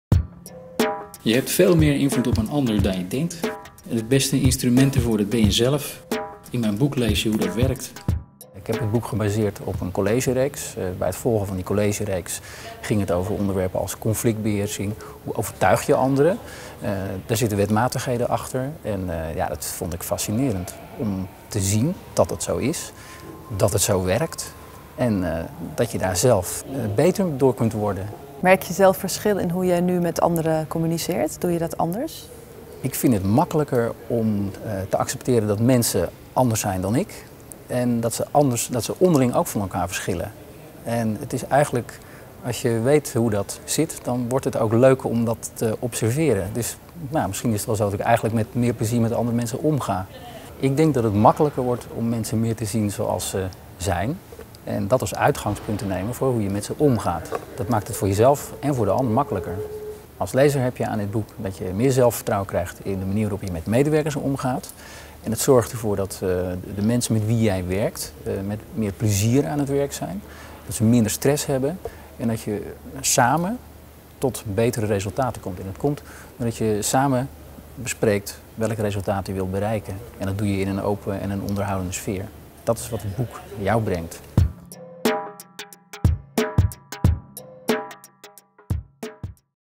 Toine Al over psychologie voor managers
Vraag je je ook wel eens af wat medewerkers en collega's echt denken? Waarom jouw gedrag een heel ander effect heeft dan je bedoelde? Dan ben je de enige niet. Toch is het gedrag van de mensen met wie je dagelijks te maken hebt beter te voorspellen dan je denkt. Als je weet welke mechanismen erachter zitten.

Toine Al zet de meest voorkomende op een rij, wanneer je ermee te maken hebt, hoe ze werken en hoe je er mee om kunt gaan. Hij baseerde zich op de succesvolle collegereeks van Focus Conferences: 'Psychologie voor Managers', die gegeven wordt door bekende hoogleraren als Ap Dijksterhuis, Margriet Sitskoorn, Carsten de Dreu, Theo Compernolle en Rob Vinke.

'Een update van je mensenkennis, voor iedereen die leiding geeft
(...en krijgt)'.

Psychologen in dit boek:
'Feedback is ook belangrijk als alles goed gaat'
Roos Vonk
'Dat je alles in de hand hebt is een fabeltje'
Ap Dijksterhuis

Leidinggevenden in dit boek:
'Het helpt me veel effectiever te managen'
Bart Kruyning, Ballast Nedam Bouw & Ontwikkeling Zuid
'Voorkomen dat je puur afgaat op je intuïtie'
Kees Bruin, directeur VMBO/MBO-school